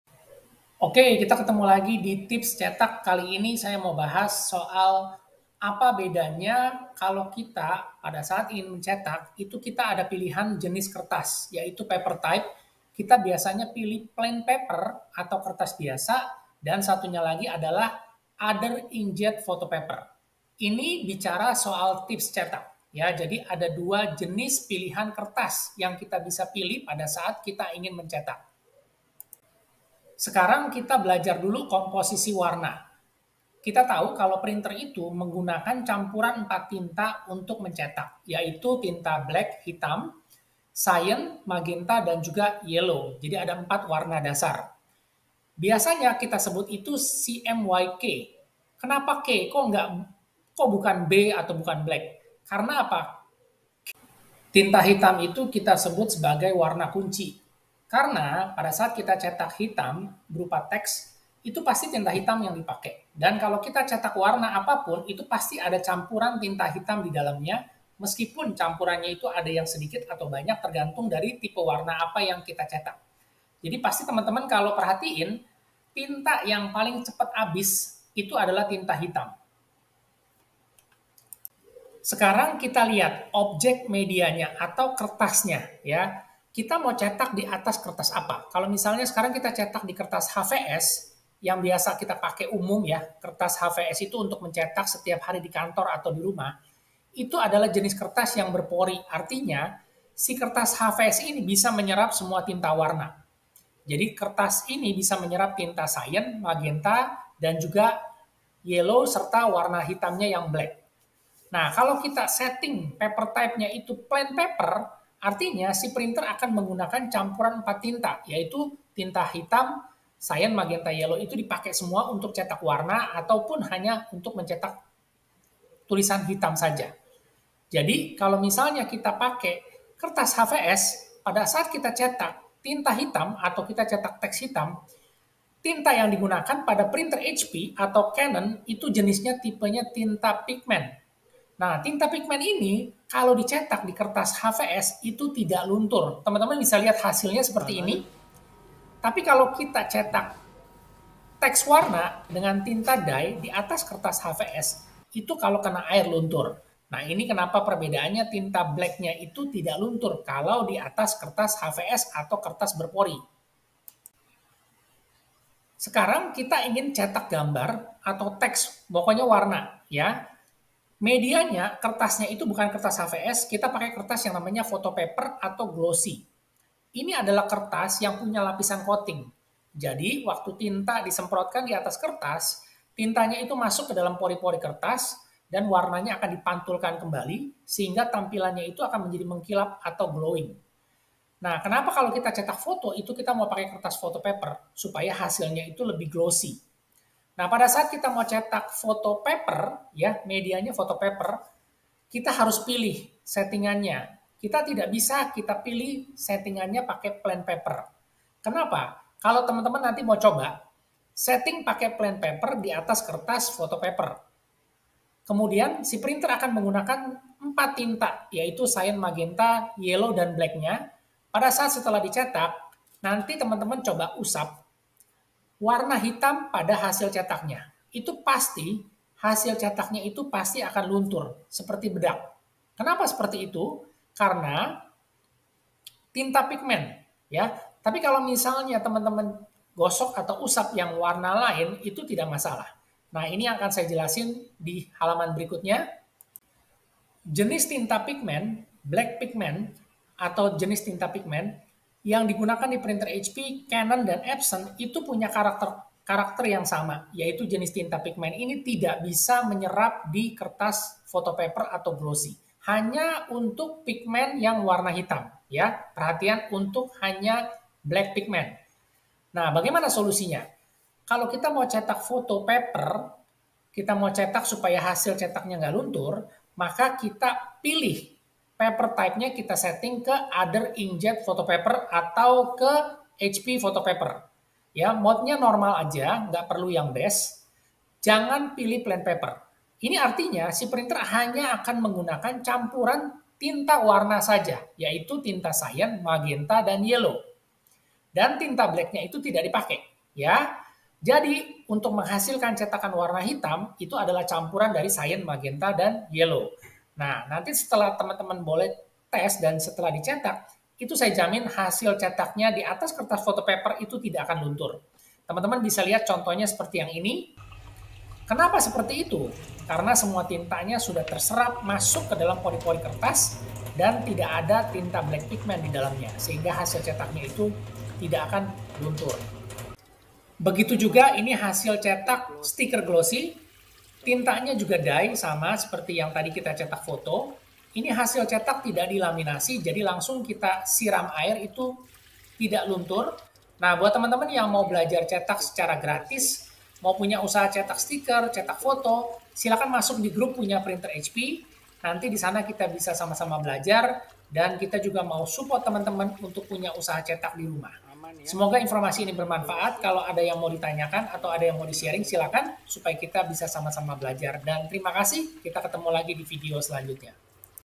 Plain Paper vs. Other Inkjet Photopaper
Buat Teman2 pengguna Printer HP dan Canon,  yang masih bingung, Kok Cetak Gambar/Foto pada media kertas Photopaper/Glossy, hasil cetaknya Luntur?  Terutama Warna Hitamnya kok di usap jari aja langsung luntur

Disini kita sharing soal apa bedanya pada saat kita mau cetak, itu ada Pilihan Paper type yg di sediakan di Printer Properties nya, ie. Plain paper, Photopaper, Matte, Glossy, Other Inkjet Photopaper, dll
Ini fungsinya apa?

Nanti kalau sudah nonton video, silahkan di Test Print yah. dan Comment, Hasil nya bgmn?

** Kalau ada pertanyaan yg belum di jawab, Bisa comment atau tanya langsung di:

Order printer bisa WA : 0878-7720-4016 (KHUSUS Order Printer ya)